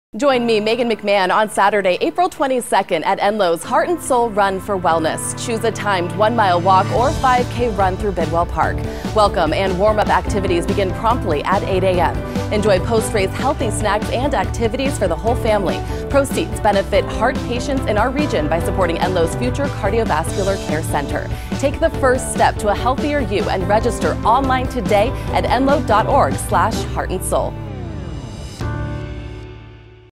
Enloe's Heart And Sole Event 2017
Enloe Medical Center's third annual Heart & Sole: Run for Wellness is April 22 at Bidwell Park's One Mile Recreation Area in Chico.

Participants can choose between a 1-mile walk and a 5-k run and help heart patients at the same time. Proceeds benefit Enloe's future Cardiovascular Care Center.